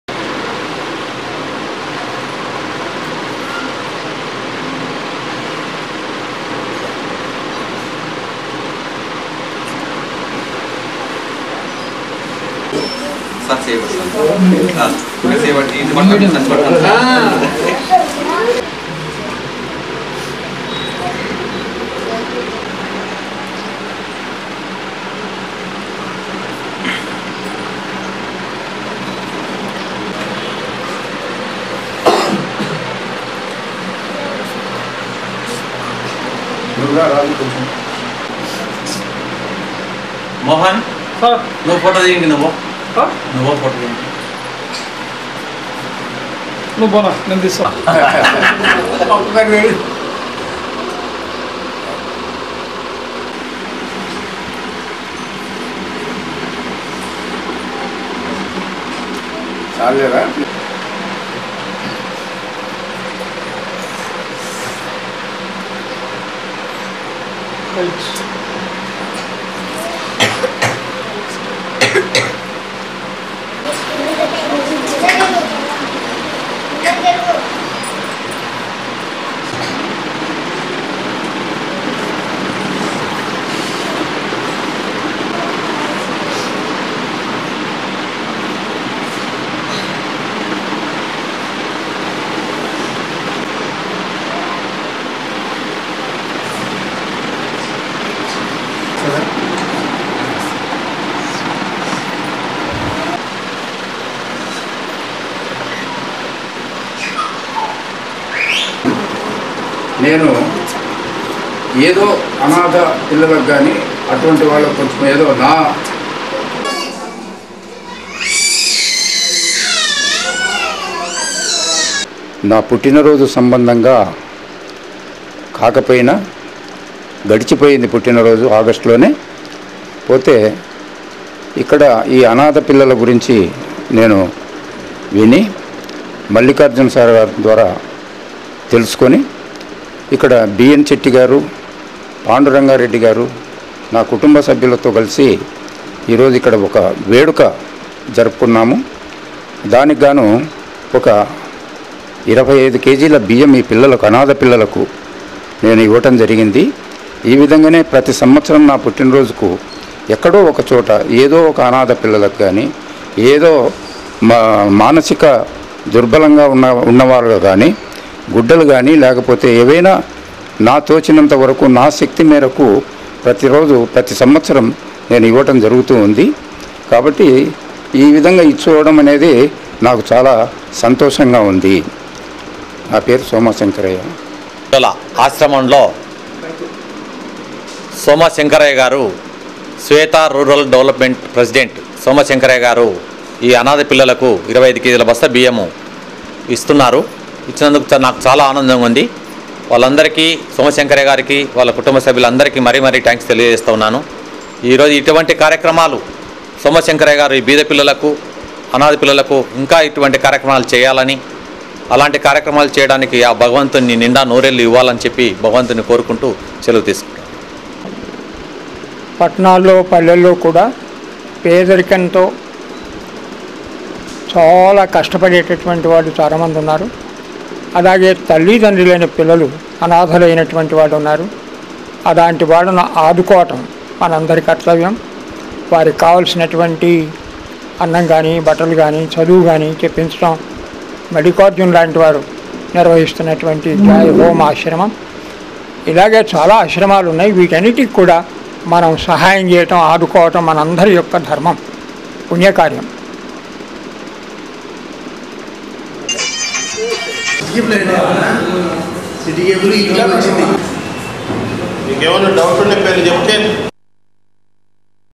Rice bag donation to Orphan Home in Kurnool | SERUDS NGO
Mr.Somashankaraiah a retd. head master donated a 25Kgs Rice Bag, Sweets distribution to the Orphan children in Joy Home Children Orphanage in Kurnool which runs by SERUDS NGO India on 15th October 2013. The donor has donated this rice bag on the occasion of his birthday. Mr.B.N.Chetty, Environmentalist & District Observer - MoEF, Kurnool and Ex.President of Lions Club Mr.Prabhakar Reddy has visited the Joy Orphan Home in Kurnool. Orphan Children were very happy for this kind support. If you would like to know more about SERUDS NGO ongoing projects please